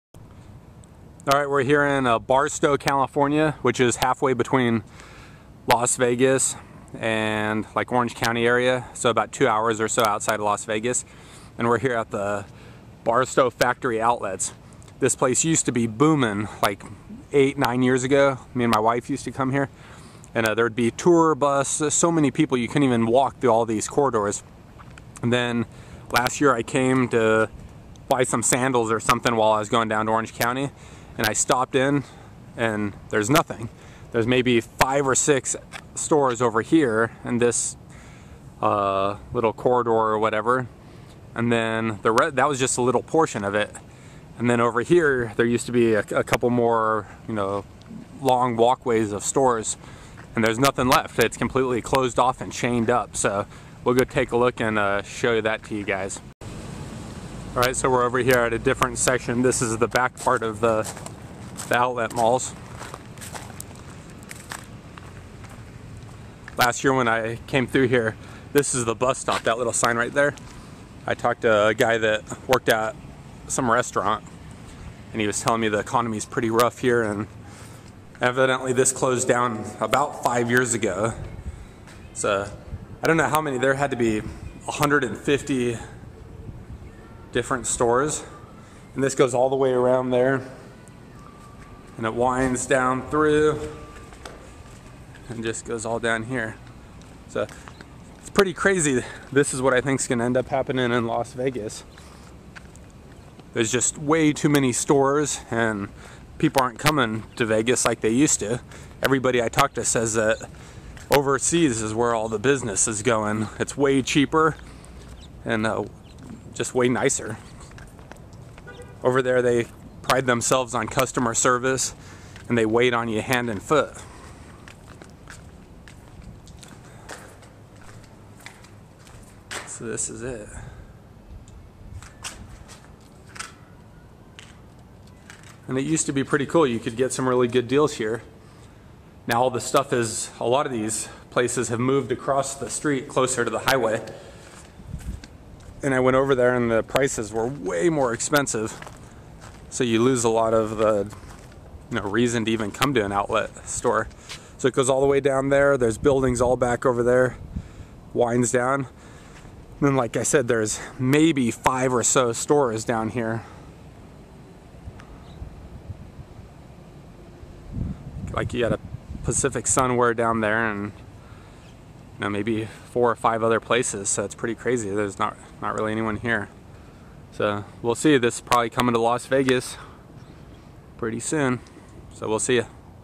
Barstow outlet malls
Quick tour of the near ghost town aka Barstow Outlet Malls.  This place was the happening stop between Las Vegas, NV and Southern California in the late 90's and early 2000's.  I stopped through and couldn't believe the condition of the place.